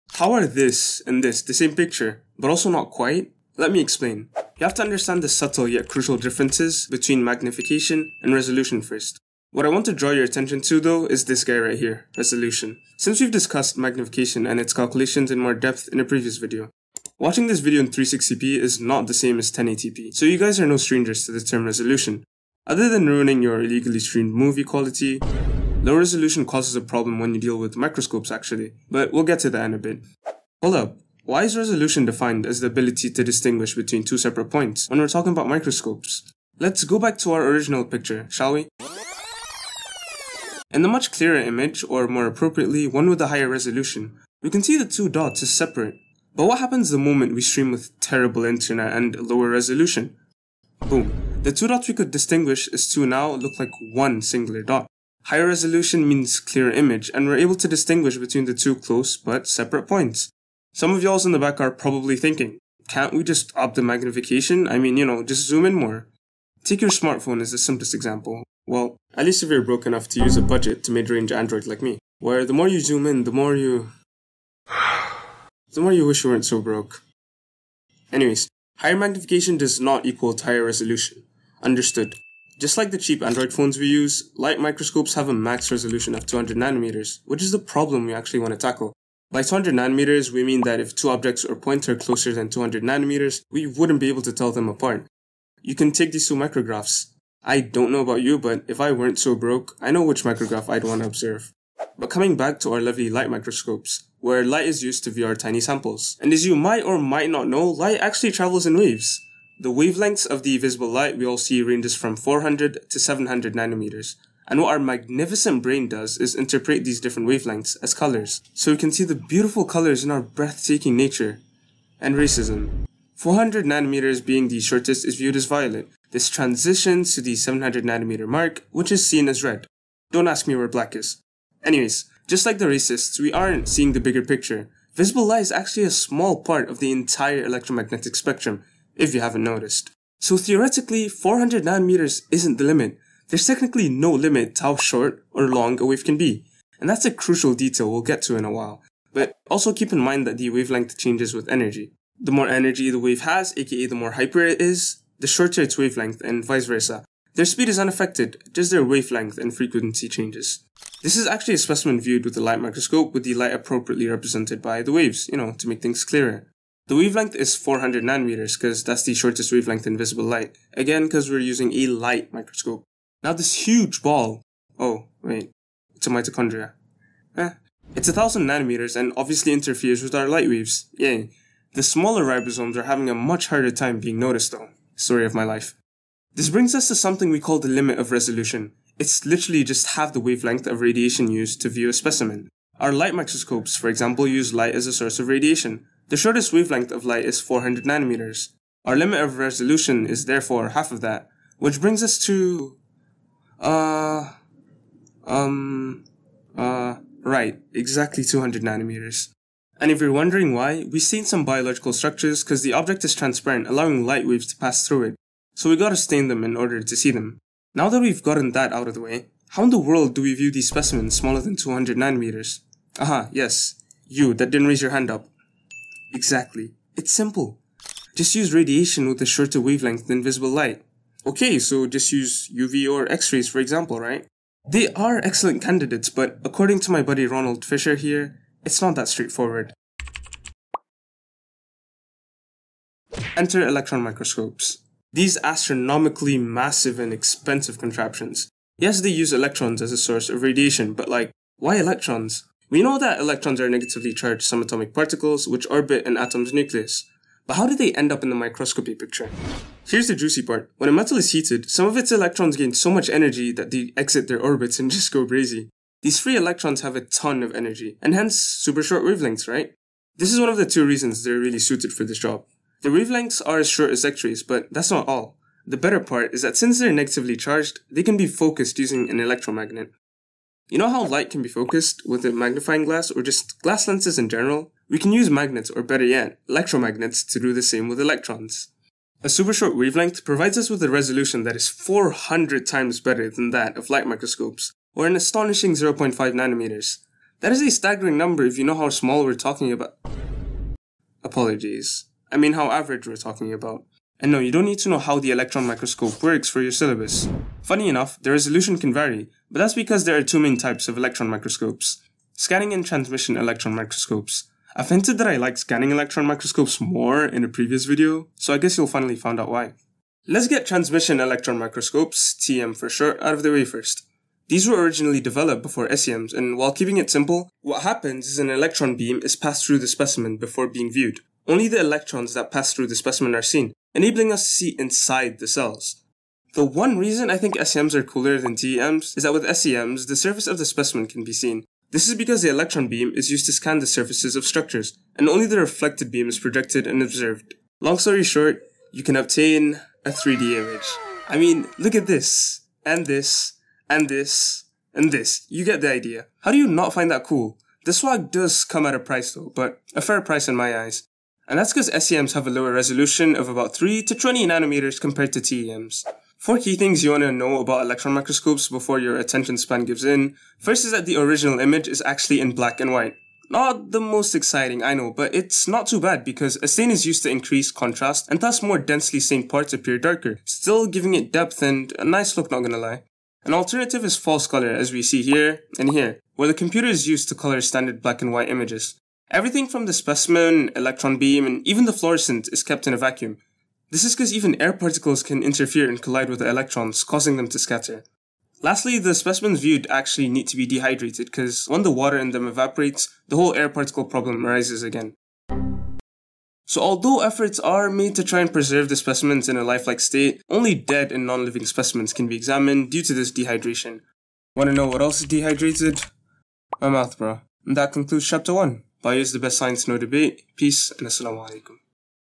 A Level Biology Cell Structure (Chapter 1) | Part 5 | Cambridge
Infinite Magnification with microscopes sounds cool but its really all for nothing without this ONE key element; Resolution. A-level biology won't let you slide without it.

🤔🤔Wonder what makes the 2 images different but the same??? Watch the video to find out as we uncover the differences between Magnification and Resolution, as well as their importance in Electron Microscopy. 🤔🤔

Idk what to include in the description so ask anything you need in the comments and I'll be super pleased to respond.

Stampssssssssss: